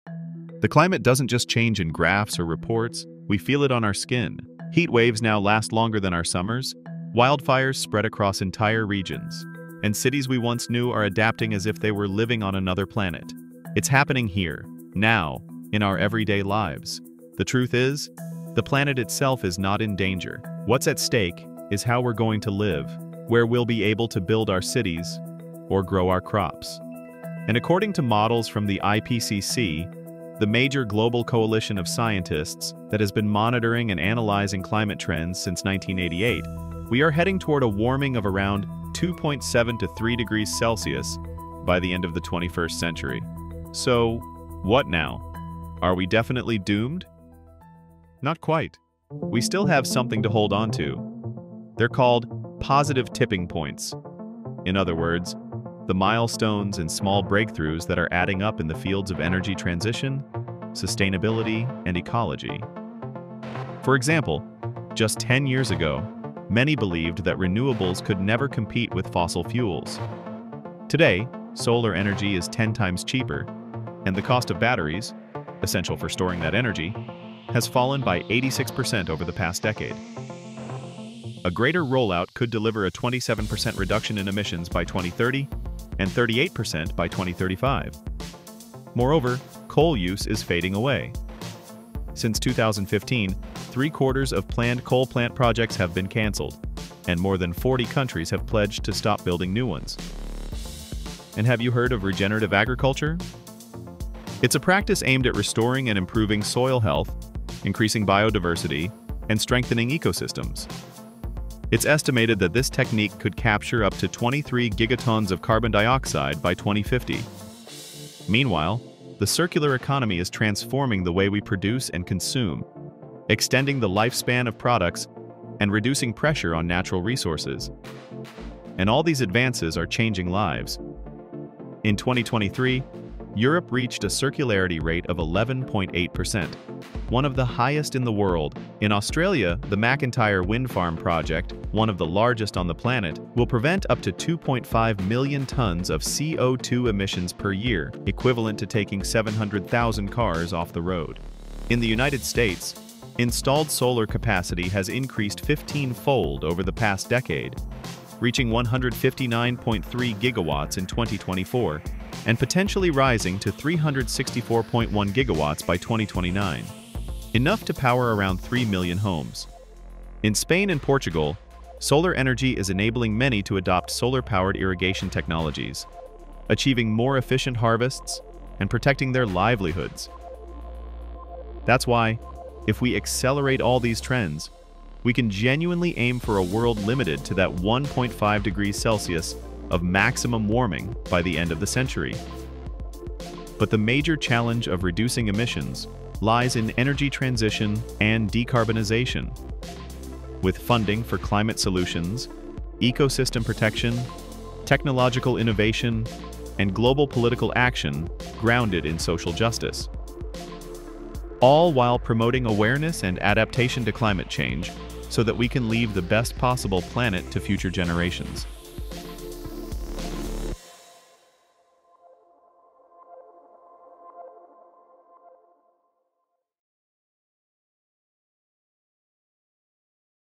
A Positive Outlook on Climate Change | 2025 International Day Of Climate Action
Climate change poses challenges—but also opportunities. And there are solutions and positive milestones, or "tipping points," concentrated around the energy transition, sustainability, and ecology. From the plummeting cost and unstoppable growth of clean energy, to the transformation driven by the circular economy and the rise of regenerative agriculture—these shifts are already changing lives.

If we accelerate all these trends, we can aim for a world that is not only adapted to climate change but also capable of slowing global warming and reducing its worst impacts. To achieve this, a sustained and fair energy transition and decarbonization are critical—supported by increased funding for climate solutions, the protection of ecosystems, the promotion of technological innovation, and bold global political action rooted in social justice.

Don’t miss this video—and share it with those you care about to keep raising awareness about climate change.